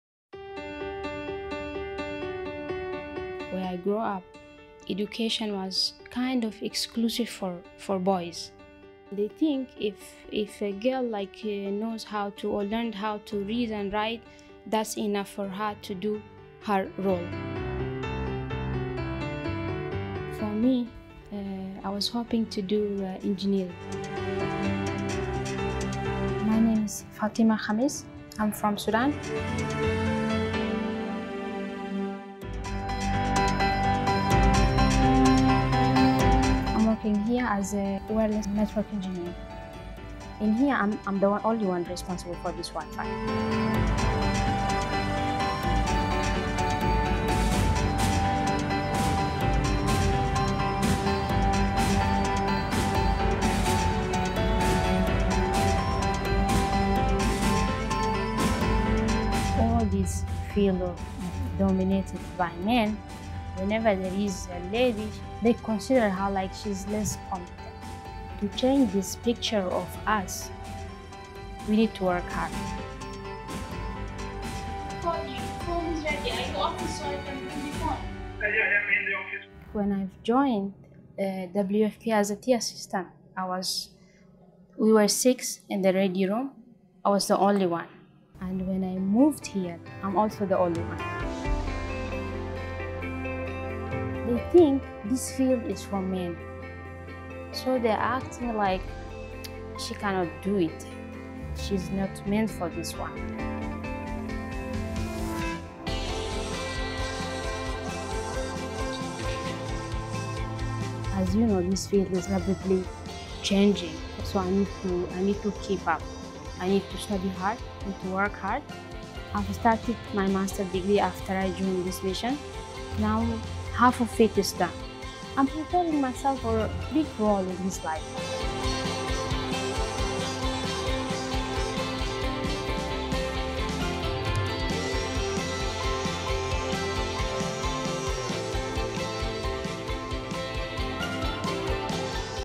République centrafricaine : une femme pionnière dans les nouvelles technologies
Fatima Khamis est une femme extraordinaire. Après s'être opposée à sa famille dans son pays d'origine, le Soudan, pour étudier l’informatique, elle s'est rendue en République centrafricaine (RCA) où elle a rejoint la Mission de maintien de la paix des Nations Unies, la MINUSCA. Elle nous raconte son histoire et ce que cela fait d’être la seule femme travaillant dans le domaine des technologies sans fil au sein de la mission.